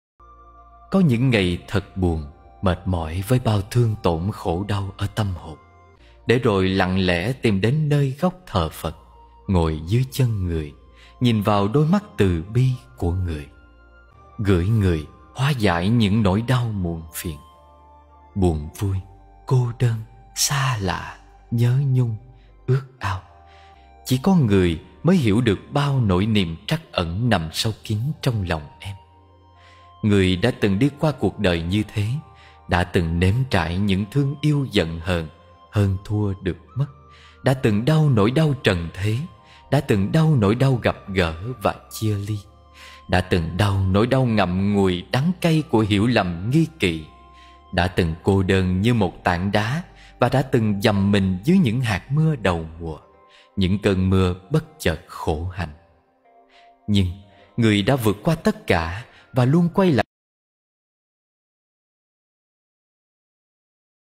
ClA Có Những Ngày
Mến chúc SK

Lời khấn nguyện
  Cha Ngọc Đế.Mẹ Phật Mẫu Diêu Trì

Kính lạy Cha Ngọc Hoàng🤩
Kính lạy Mẫu Diêu Trì
Con nay xin khấn nguyện
Với ngôi Trời bao dung 😍
Vì vô Minh che phủ
Kiếp Luân hồi trả vay
Cha từ bi thứ tội
Mẹ rộng lòng bao dung 😍